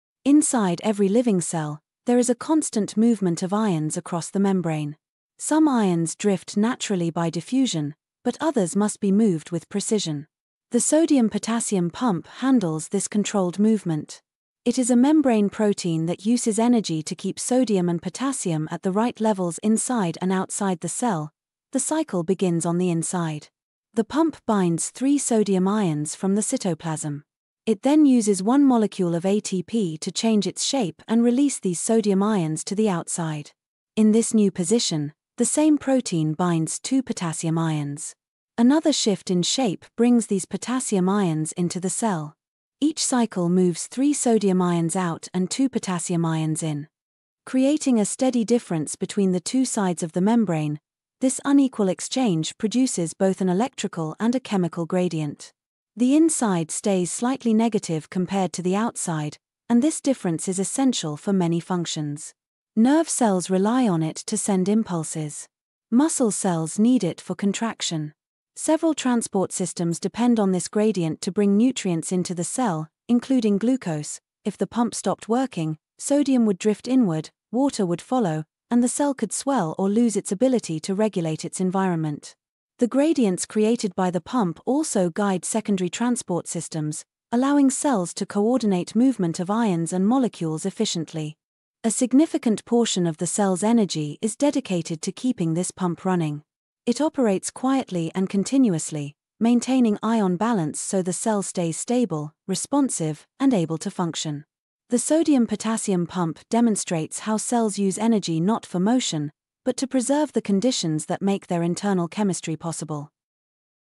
The Sodium-Potassium Pump: How Cells Use Energy Without Moving
Inside every living cell, a silent engine runs nonstop — the sodium-potassium pump. This membrane protein doesn’t power motion, but something deeper: the chemical conditions that make life possible.

In this short, discover how the Na⁺/K⁺-ATPase uses ATP to maintain ion gradients, enabling nerve impulses, muscle contractions, and nutrient transport. Learn why cells dedicate so much energy to this quiet process — and what happens if it stops.